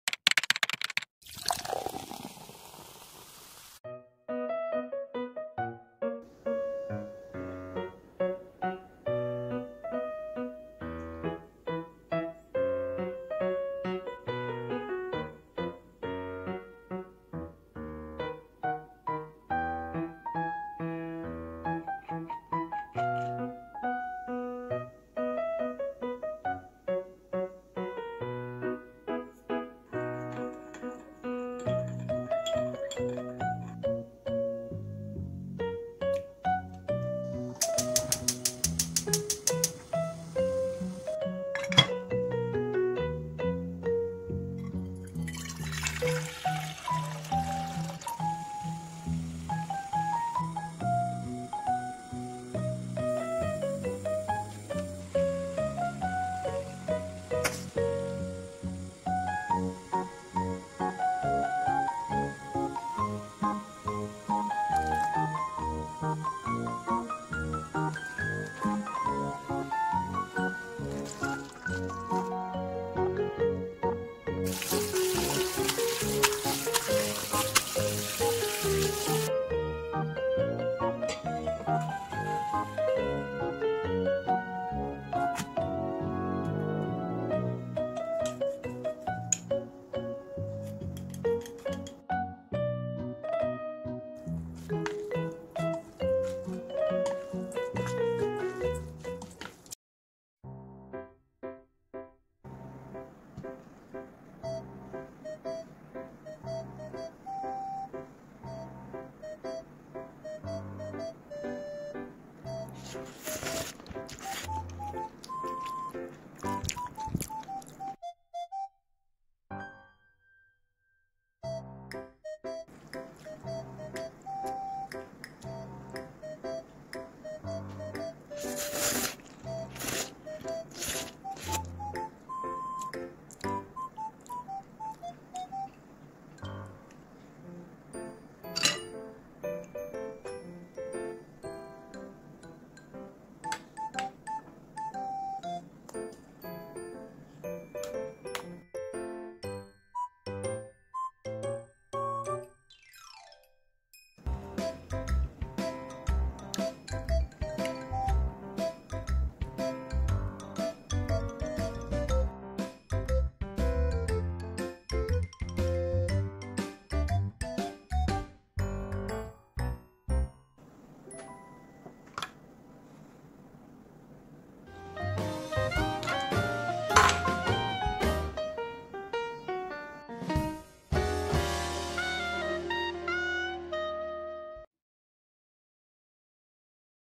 (ENG SUB)●One meal today ●Simple cooking●Soy sauce Bibimguksu#04●Korean food
Material]
- Noodles, marinade.

[Laughs]

- soy sauce2, sugar1, red pepper powder, sesame oil, salt, sesame seeds, 0.5

[Method]

1. Make a marinade.

2. Boil noodles in boiling water for 3-4 minutes.

3. Cool the noodles in cold water, drain and pour the marinade.

4. Mix it and eat it deliciously.